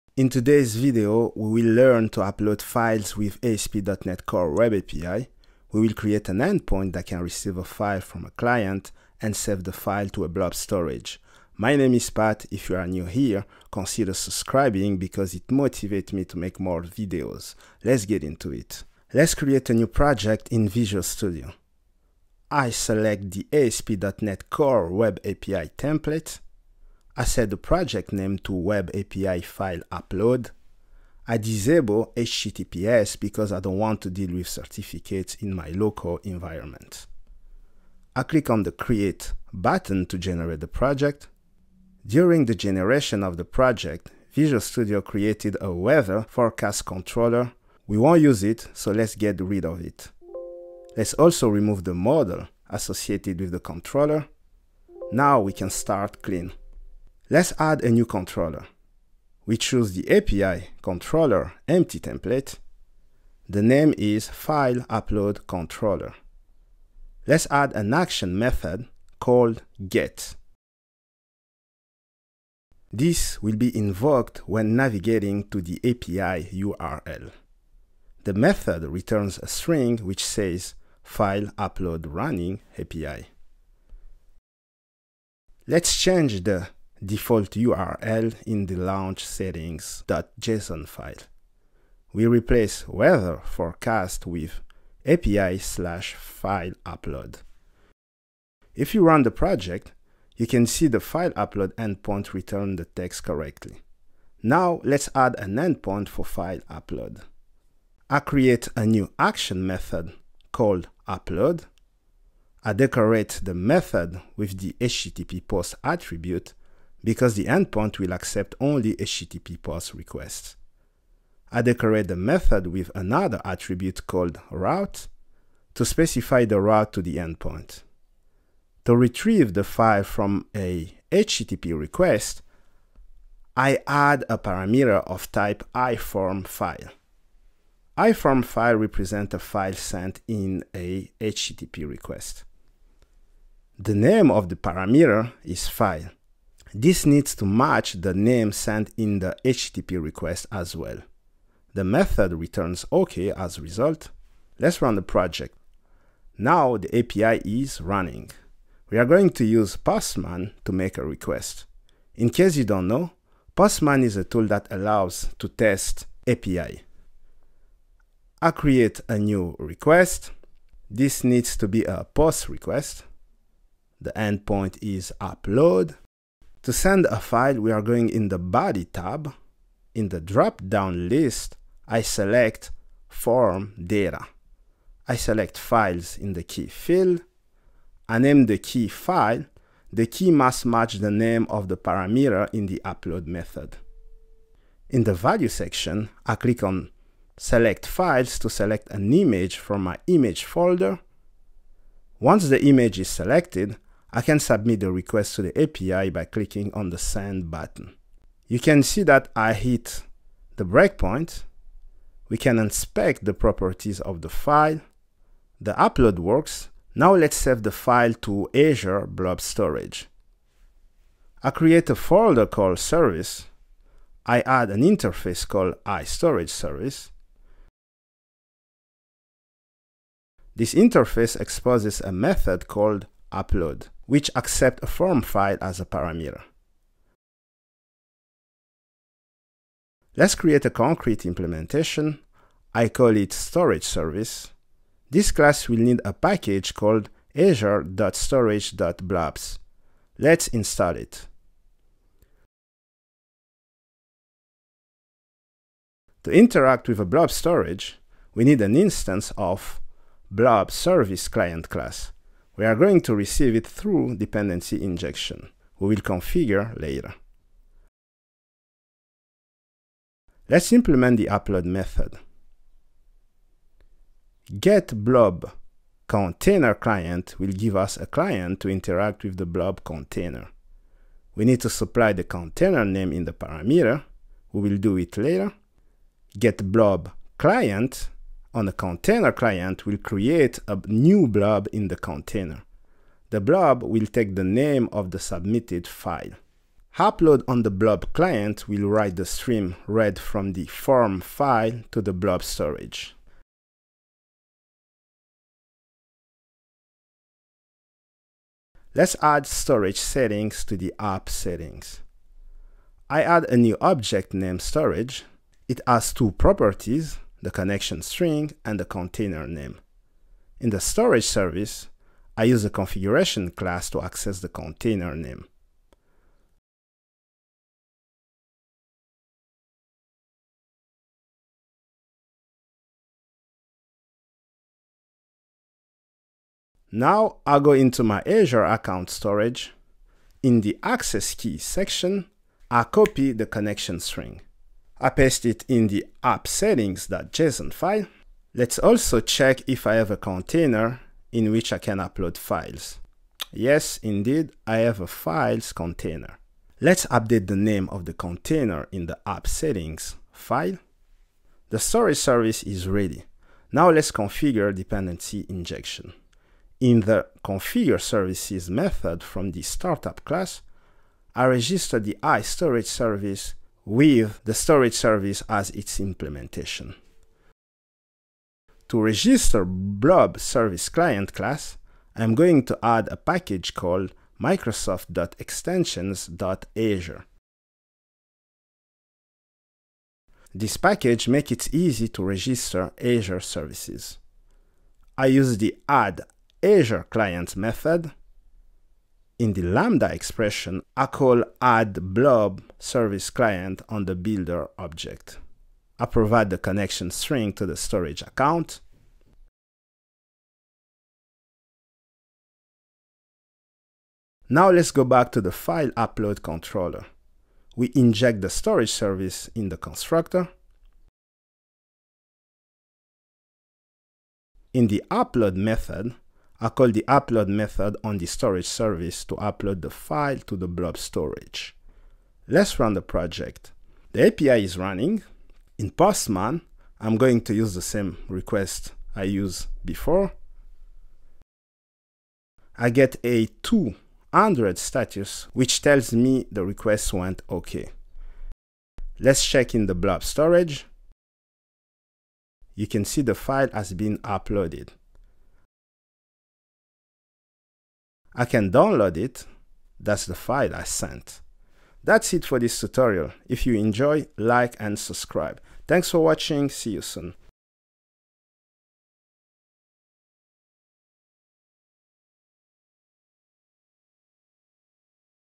How to UPLOAD file with ASP.NET CORE WEB API | C# tutorial for beginners
In this video, we will learn to upload files with ASP.NET Core Web API.
We will create an endpoint that can receive a file from a client and save the file to a blob storage.

🕑 Timestamp :
00:00 Introduction
00:18 Creating an ASP.NET CORE project in Visual Studio
01:47 Implementing File Upload
03:41 Saving file to Azure Blob Storage
07:42 Outro